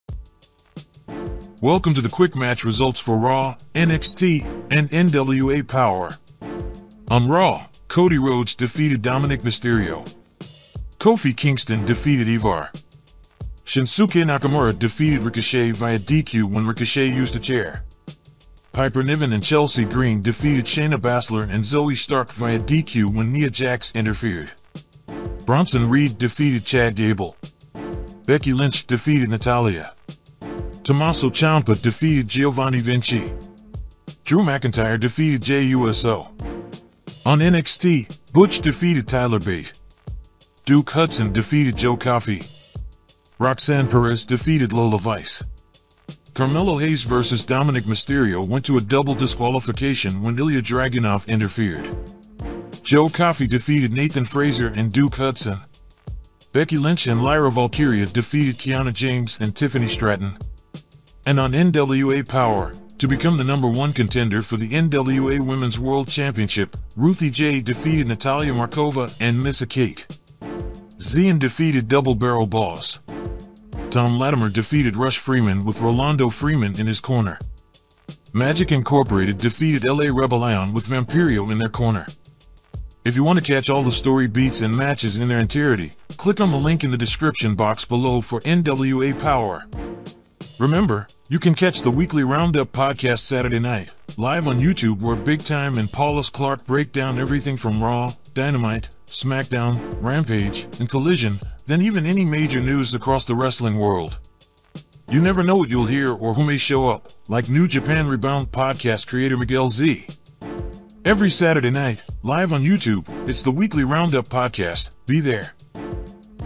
Quick Match Results | Raw, NXT, and NWA Power | 9/19/2023 and 9/20/2023
Quick match results for this week's Raw, NXT, and NWA Power shows.